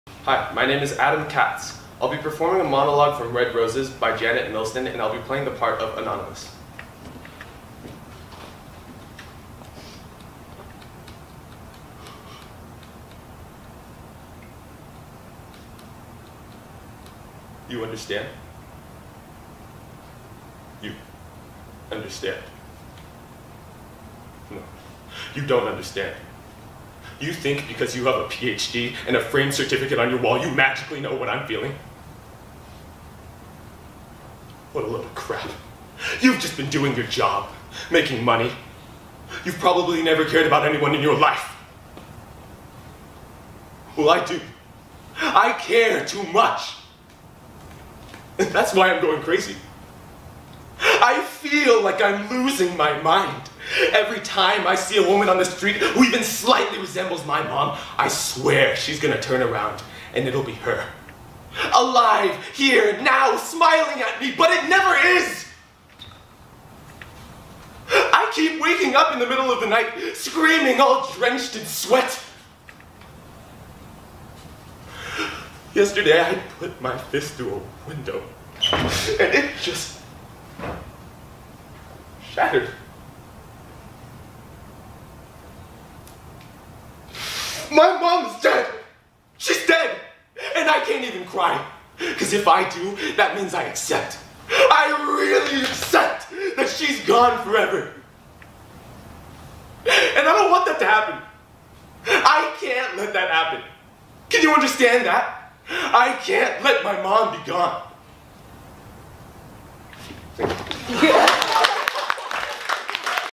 Roses are Red (Craig) DRAMATIC MONOLOGUE
Hi my name is Adam. I found this monologue on the internet and really enjoyed it. I performed it in my drama class and loved it.

I just want to share it with everyone on here so that you all can use the monologue too if you want to too.